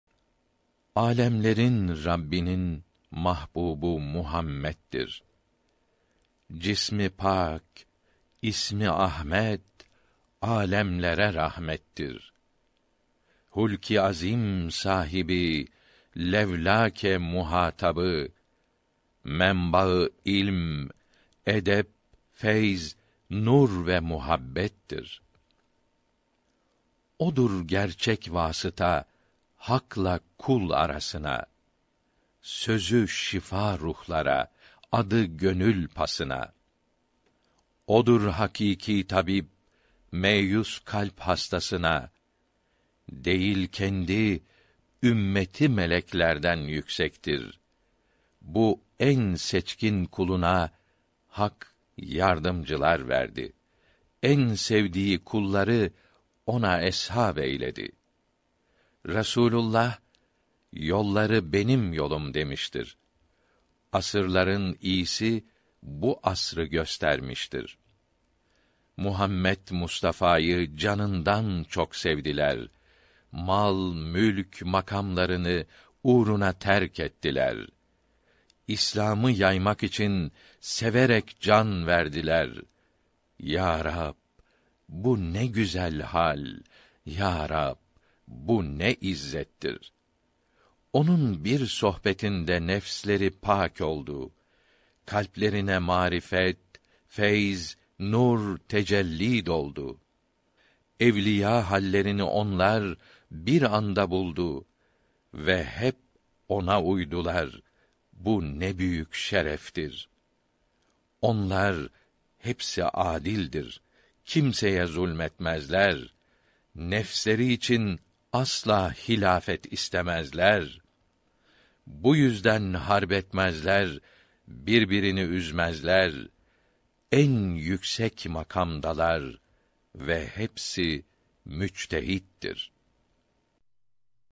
Hakikat Kitâbevi Eshab-i Kiram Şiir
Âlemlerin Rabbinin mahbûbu Muhammeddir.
Cismi pâk, ismi Ahmed, âlemlere rahmetdir.

Hulk-i azîm sâhibi Levlâke. muhâtabı,
Menba-ı ilm, edeb, feyz, nûr ve muhabbetdir.

Odur gerçek vâsıta, Hak’la kul arasına,
Sözü şifâ rûhlara, adı gönül pasına.

Odur hakîkî tabîb, me’yûs kalb hastasına,
Değil kendi, ümmeti, meleklerden yüksekdir.

Bu en seçkin kuluna, Hak yardımcılar verdi,
En sevdiği kulları ona Eshâb eyledi.

Resûlullah: yolları, benim yolum demişdir,
Asrların iyisi bu asrı göstermişdir.

Muhammed Mustafâyı canından çok sevdiler,
Mal, mülk, makâmlarını, uğruna terk etdiler.

İslâmı yaymak için severek can verdiler,
Yâ Rab, bu ne güzel hâl, yâ Rab, bu ne izzetdir.

Onun bir sohbetinde nefsleri pâk oldu.
Kalblerine ma’rifet, feyz, nûr, tecellî doldu.

Evliyâ hâllerini onlar bir anda buldu,
Ve hep Ona uydular, bu ne büyük şerefdir.

Onlar hepsi âdildir, kimseye zulm etmezler,
Nefsleri için aslâ, hilâfet istemezler.

Bu yüzden harb etmezler, birbirini üzmezler,
En yüksek makâmdalar ve hepsi müctehiddir.